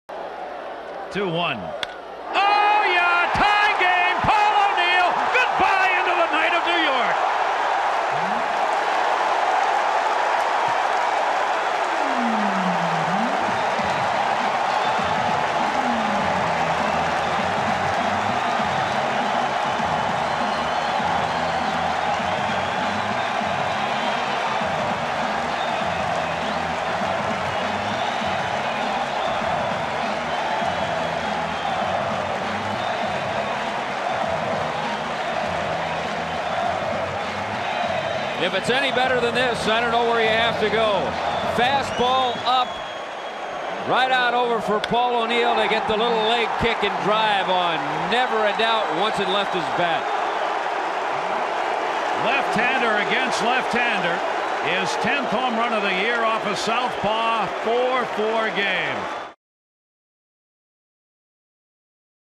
1995 ALDS Gm2: O'Neill's home run ties game
10/4/95: Paul O'Neill hits a solo home run with two out in the bottom of the seventh as the Yankees draw the Mariners even at 4 in Game 2